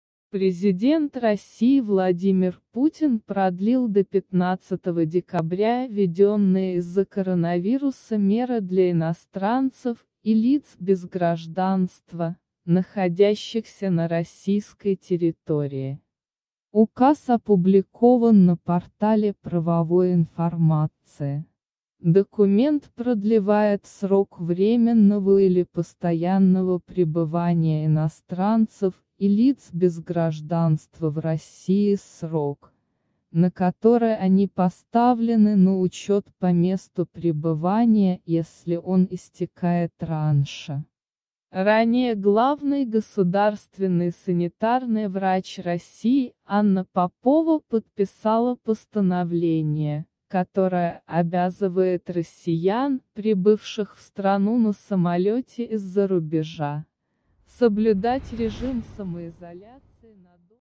СРОЧНО! Путин снова продлил сроки пребывания иностранцев в Росии
Президент России Владимир Путин продлил до 15 декабря введённые из-за коронавируса меры для иностранцев и лиц без гражданства, находящихся на российской территории.
Указ опубликован на портале правовой информации.
Документ продлевает срок временного или постоянного пребывания иностранцев и лиц без гражданства в России и срок, на который они поставлены на учёт по месту пребывания, если он истекает раньше.
Ранее главный государственный санитарный врач России Анна Попова подписала постановление, которое обязывает россиян, прибывших в страну на самолёте из-за рубежа, соблюдать режим самоизоляции на дому до получения ПЦР-теста на коронавирус.